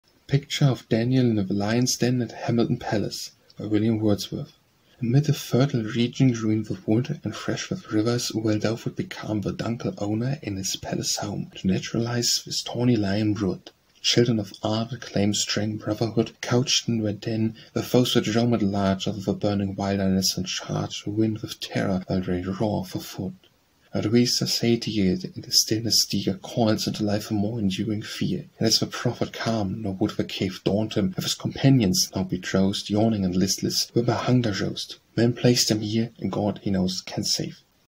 Picture of Daniel in the Lions Den at Hamilton Palace - William Wordsworth
Picture Of Daniel In The Lion's Den At Hamilton Palace

    By William Wordsworth

Amid a fertile region green with wood
And fresh with rivers, well doth it become
The Ducal Owner, in his Palace-home
To naturalise this tawny Lion brood;
Children of Art, that claim strange brotherhood,
Couched in their Den, with those that roam at large
Over the burning wilderness, and charge
The wind with terror while they roar for food.
But these are satiate, and a stillness drear
Calls into life a more enduring fear;
Yet is the Prophet calm, nor would the cave
Daunt him, if his Companions, now bedrowsed
Yawning and listless, were by hunger roused:
Man placed him here, and God, he knows, can save.